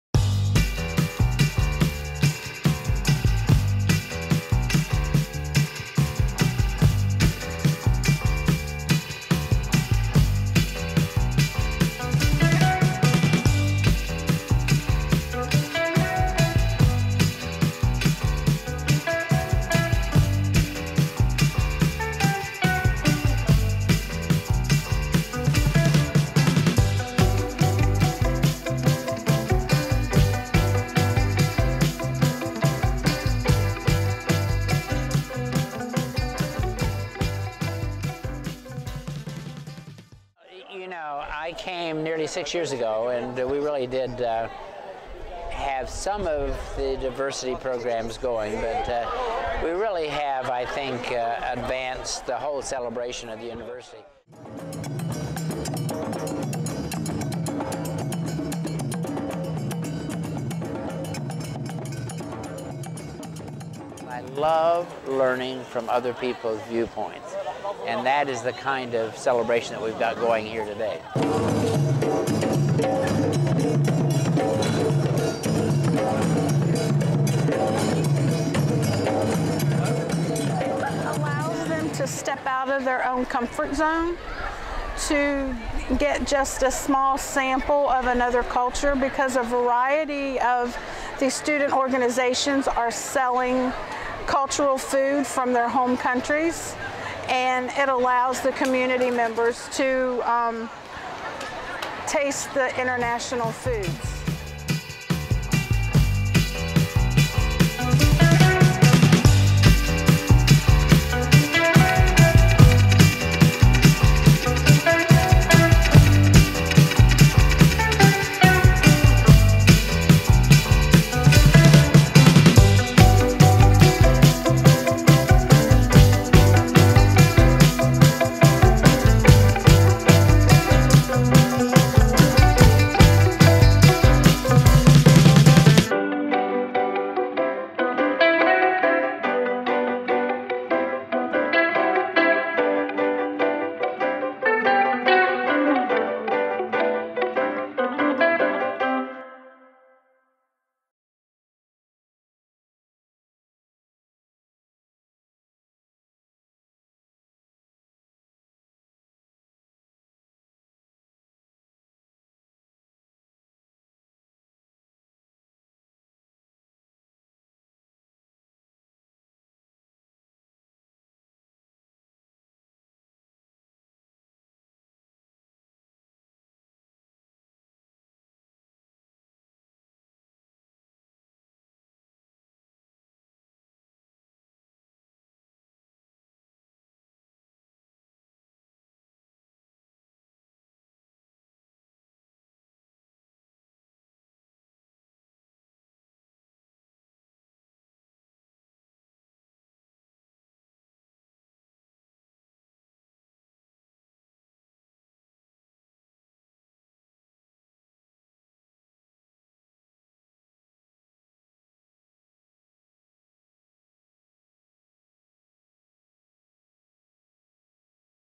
WVU celebrates cultures around the world with a street festival
To wrap up Diversity Week, WVU put on the annual International Street Festival on High Street. The event showcased various cultures, as well as different international student organizations on campus.

Video by Trenton Straight